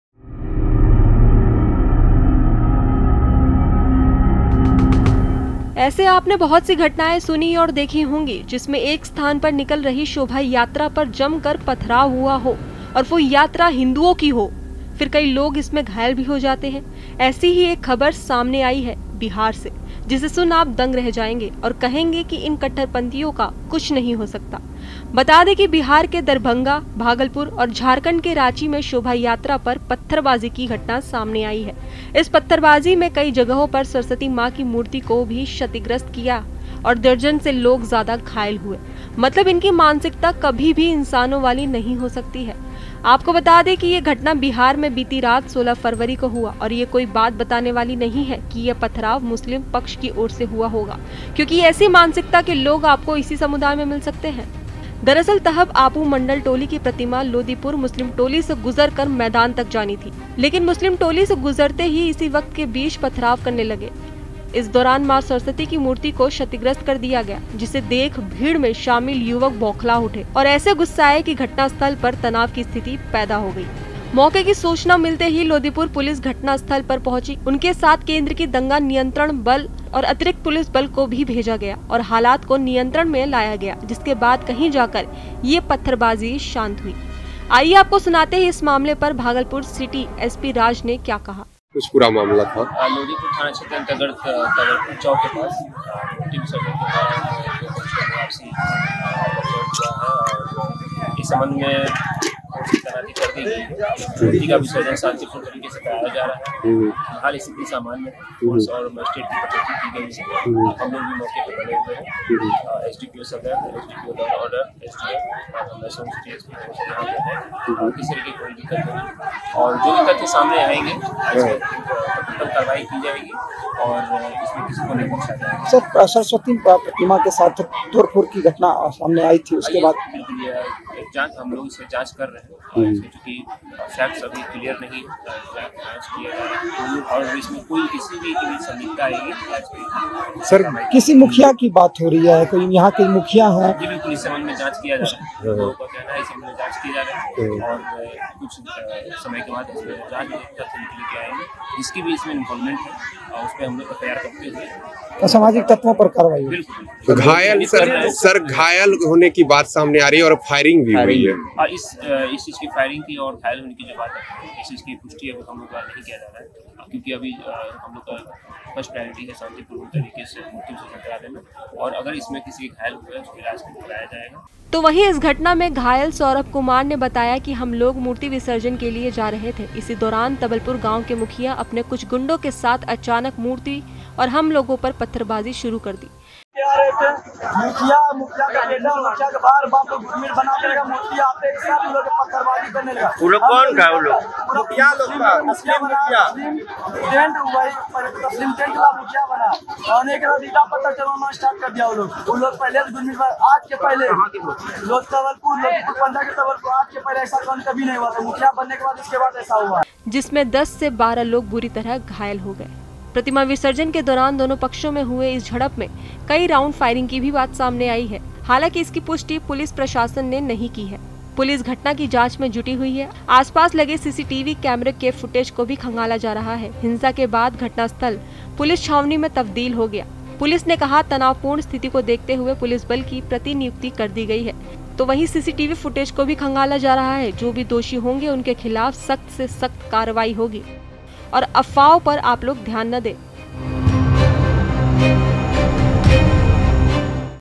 Bhagalpur में प्रतिमा विसर्जन के दौरान बवाल, पत्थरबाजी के बाद भिड़े दो गुटों के लोग | Bihar News
ऐसे आपने बहुत सी घटनाएं सुनी होंगी जिसमें जिसमें एक स्थान पर निकल रही शोभा यात्रा पर जमकर पथराव हुआ और फिर कई लोग घायल हो गए...ऐसी ही एक खबर सामने आई है बिहार से..जिसे सुन आप दंग रह जाएंगे और कहेंगे इन कट्टरपंथियों का कुछ नही हो सकता...बता दें कि Bihar के Darbhanga, Bhagalpur और JharKhand के Ranchi में शोभा यात्रा पर पत्थरबाजी की घटना सामने आई है...इस पत्थरबाजी में कई जगहों पर सरस्वती मां की मूर्ति को भी शतिग्रस्त किया और दर्जन से ज्यादा लोग भी घायल हुए ...मतलब इनकी मानसिकता कभी भी इंसानों वाली नही हो सकती है...